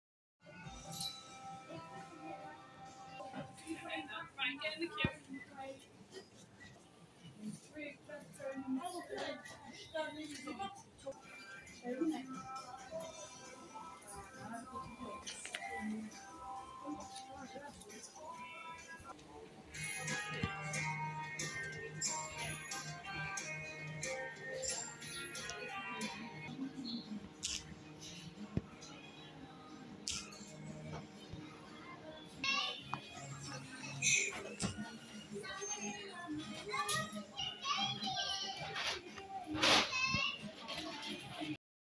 Lot's of Prime drinks at Sports Direct in Grimsby, UK
They include Girl Prime there.

Date: Today